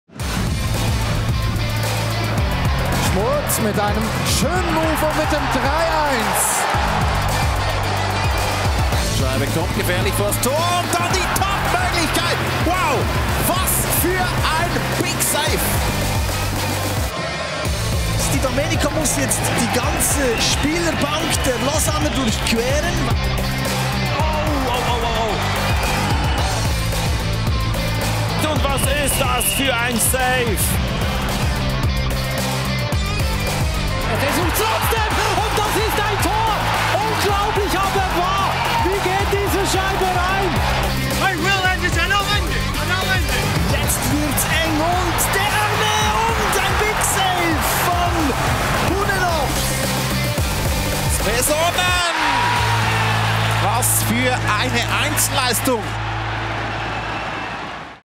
Die Saison-Highlights der SCL Tigers
Die besten Szenen aus der National League-Saison 19/20